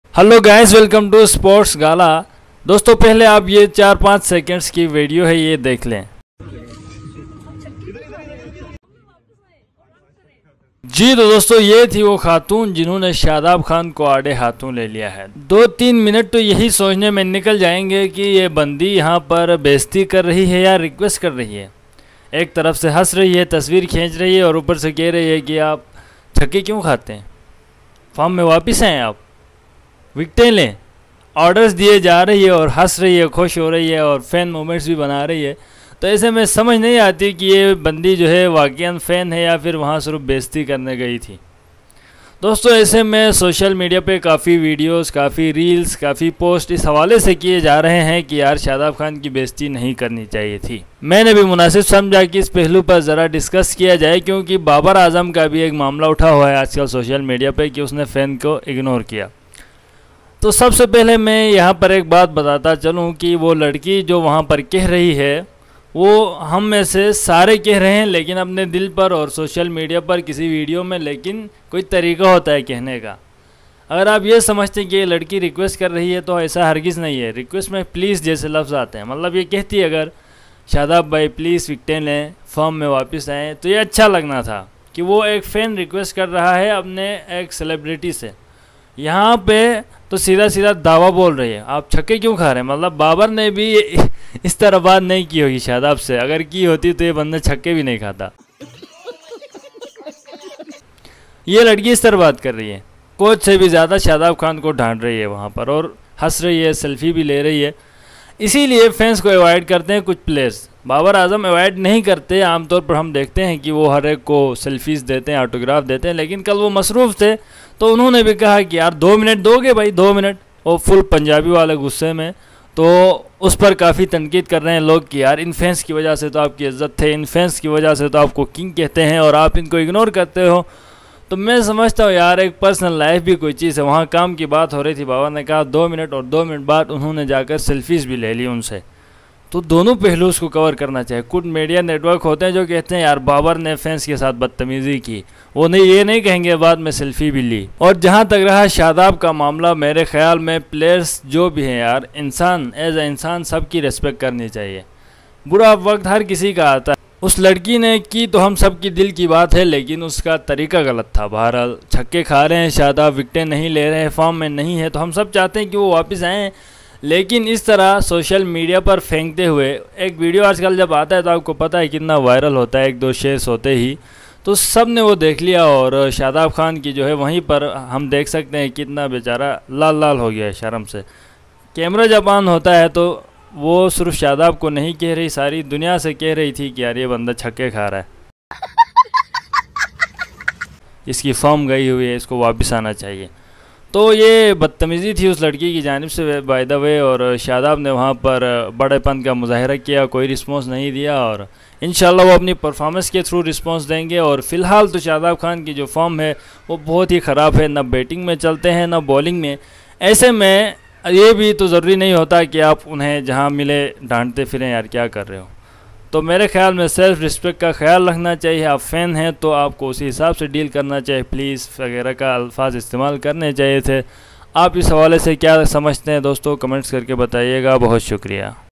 fan rude behavior with shadab khan😡 || babar azam angry on fans
Shadab Khan was insulted by his fan
A fan girl asked Shadab Khan to come back in form
shadab khan ki beizzati
shadab khan ki fan sy larai
Pakistan vs England T20 Series 2024
Shadab Khan Fan Moments
Babar Azam Fan Moments